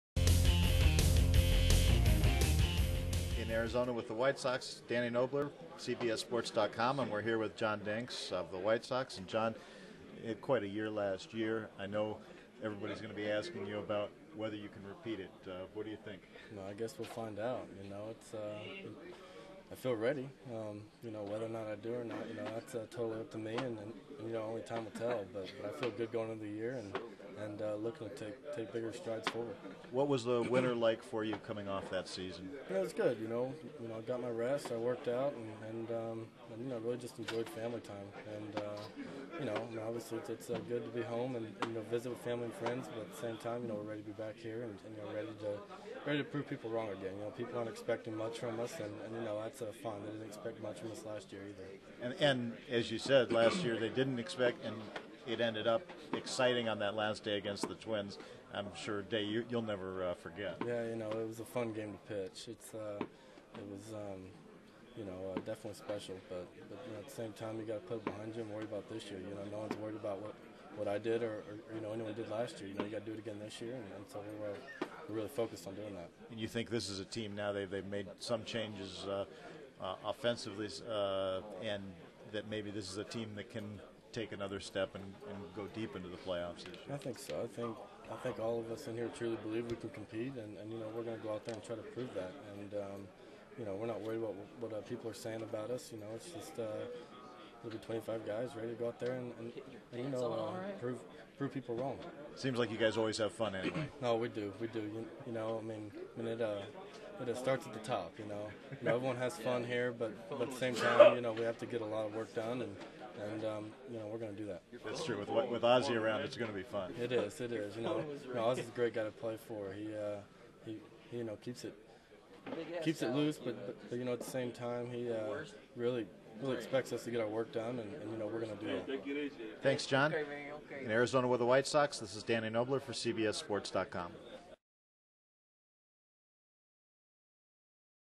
John Danks with Danny Knobler
John Danks is coming off a solid sophomore season where he went 12-9 with a 3.32 ERA. Danny Knobler had some questions for the White Sox lefty.